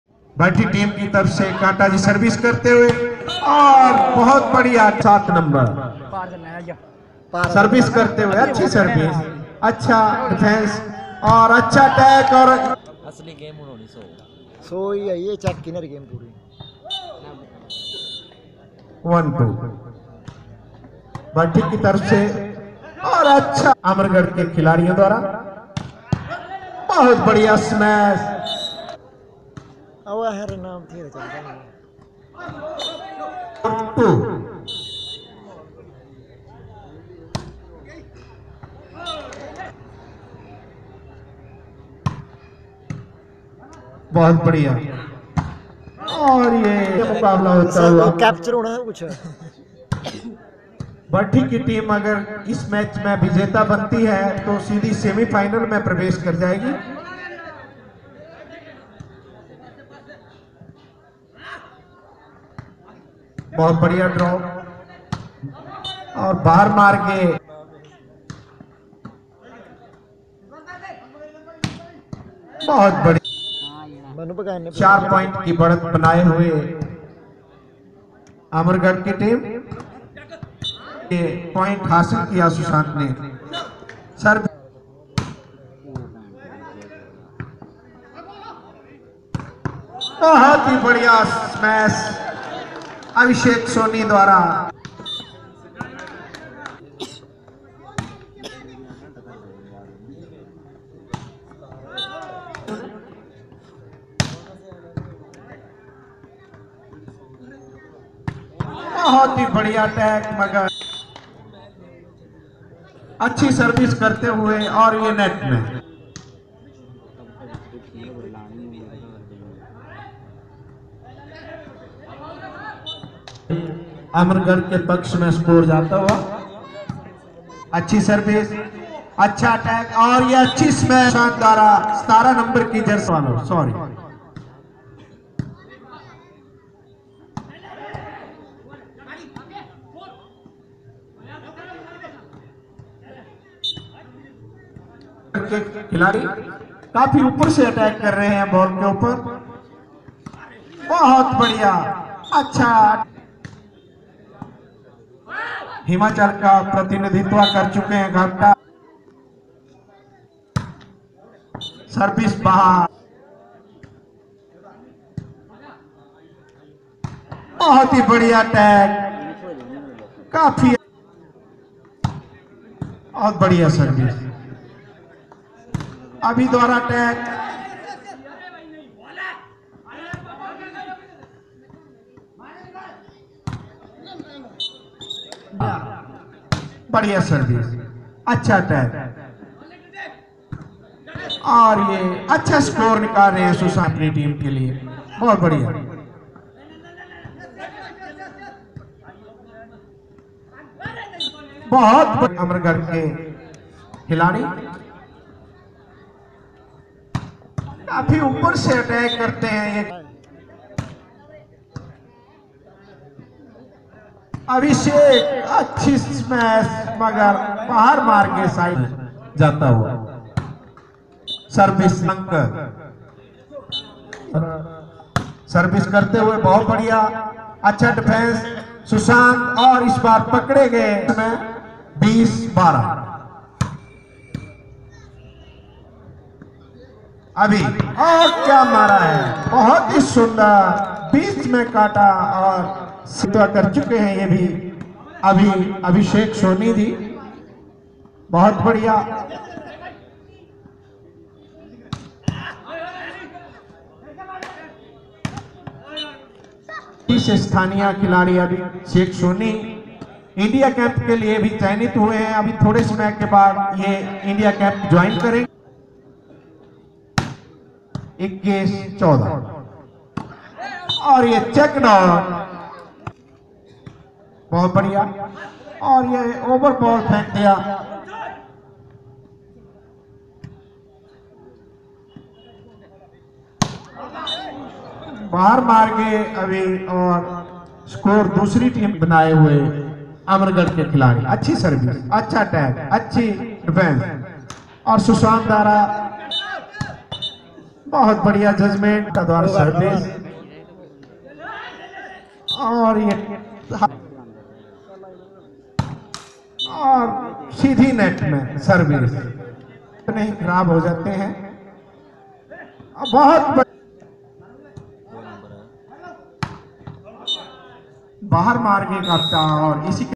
Volleyball match amargad vs berthin
Volleyball match played between amargad vs berthin